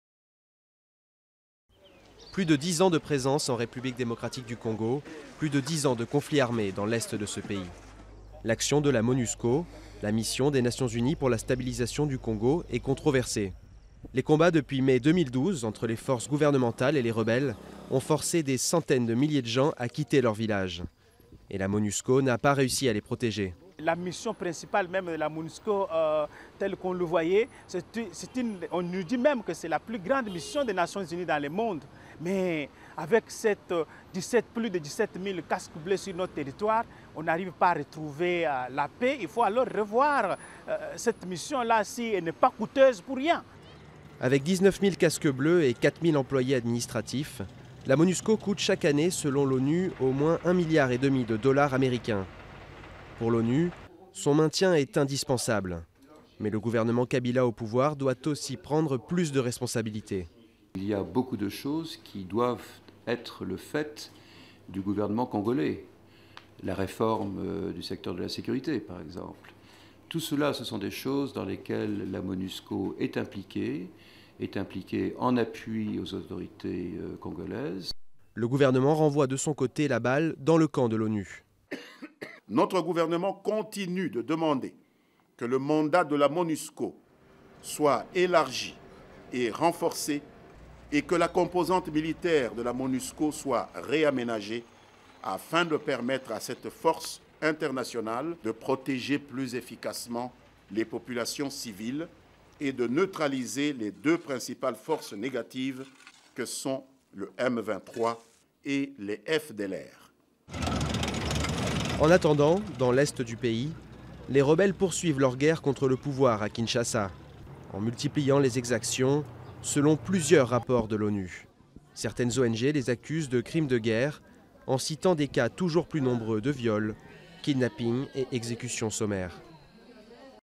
En RDC, le mandat de la Monusco remis en question
Alors que le conflit armé perdure en République Démocratique du Congo, des voix s'élèvent pour réclamer un changement de mandat de la MONUSCO, dont l'action est remise en cause après les récents affrontements entre l'armée régulière de RDC et les rebelles du Mouvement du 23 mars dans l'est du pays. Durée: 02:10